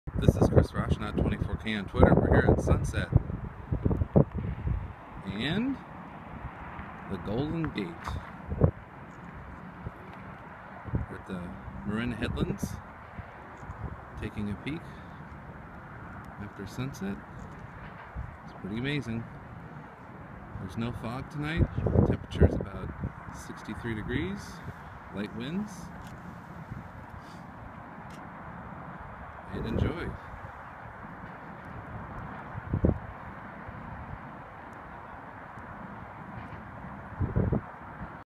Golden Gate Bridge Marin Headlands Sausalito Northern California 11-7-15
Golden Gate Bridge From The Marin Headlands In Sausalito Northern California. By Chris Rauschnot